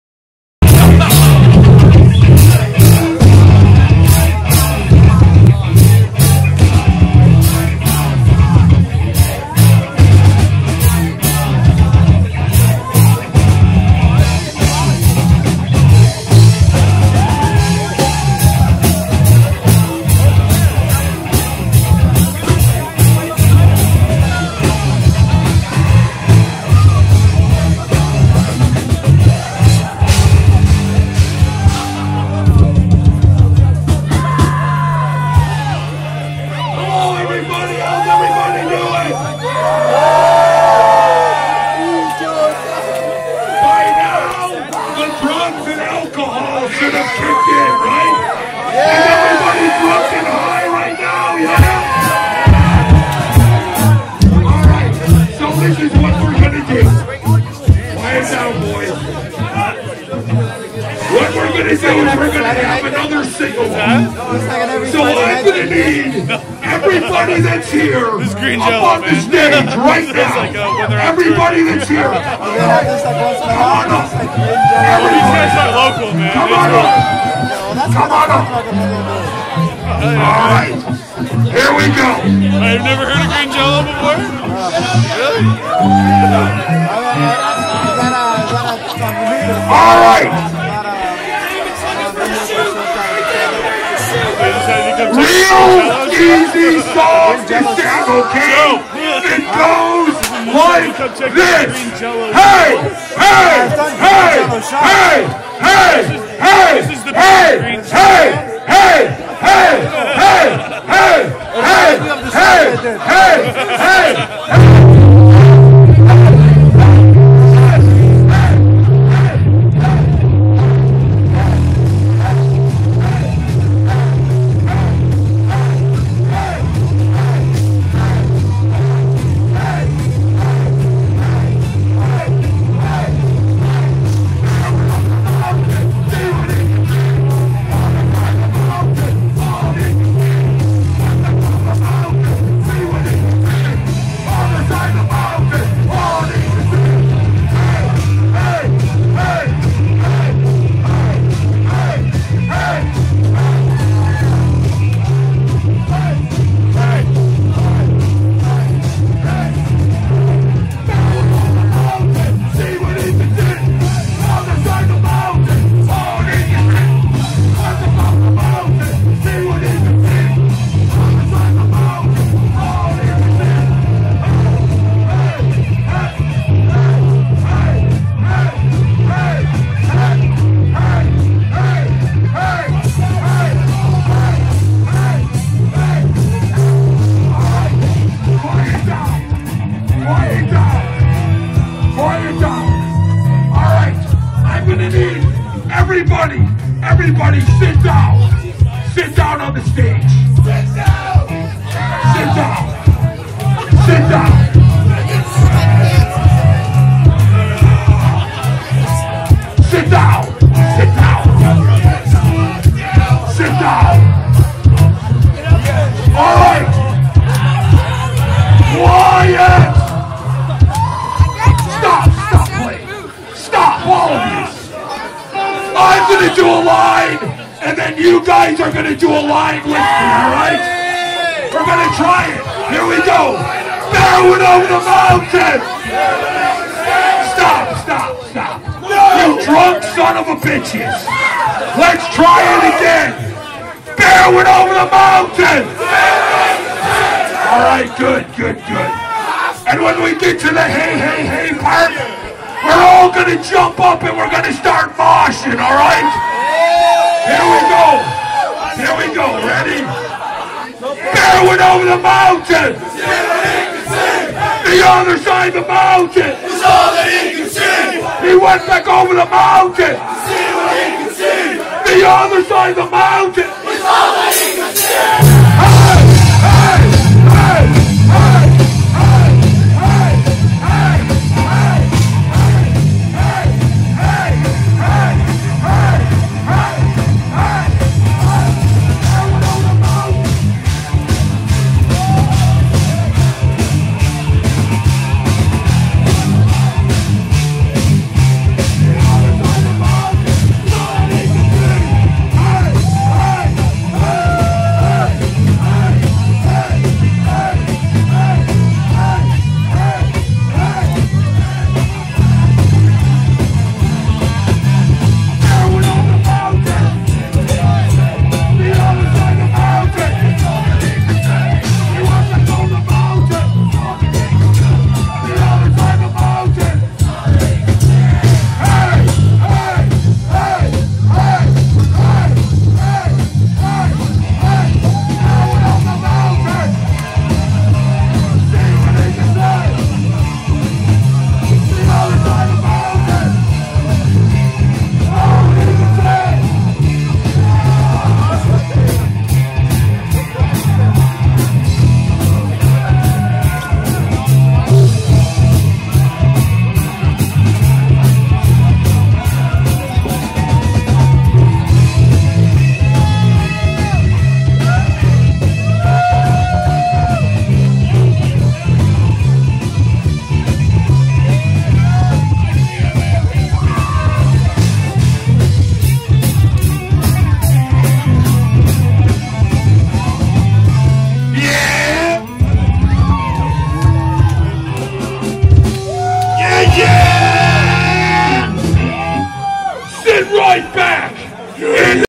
Green Jellÿ at the Atria - Nov.29, 2013 (6 of 8)
Green Jellÿ brings the Punk Rock Puppet Show to the Atria in Oshawa, Ontario!